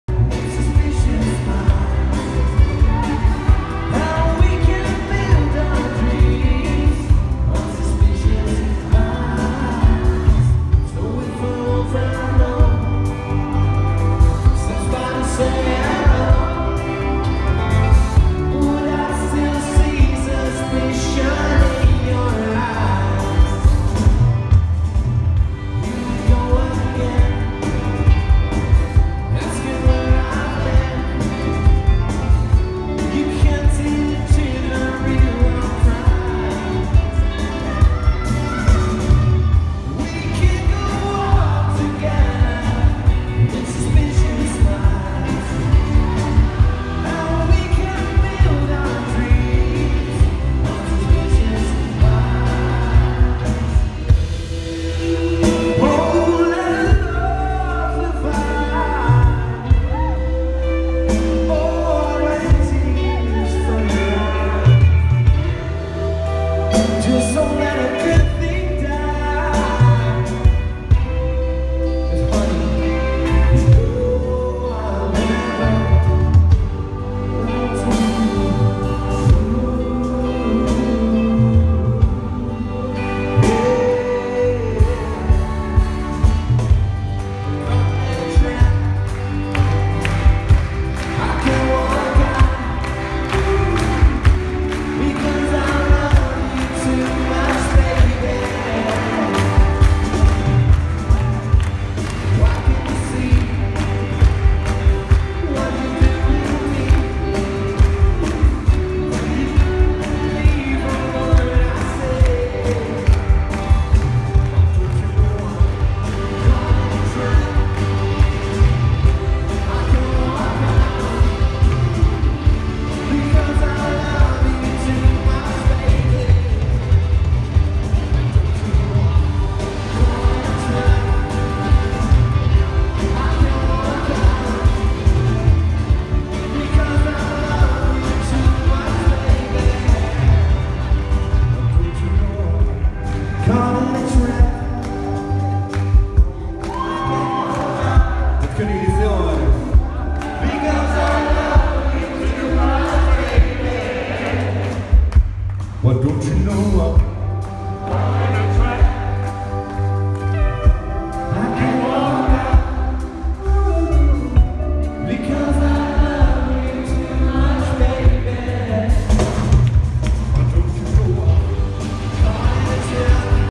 Elvis the Comeback Special - live @ Concertgebouw Amsterdam
Yorick van Norden - Lucas Hamming -Barry Hay - Anne Soldaat and more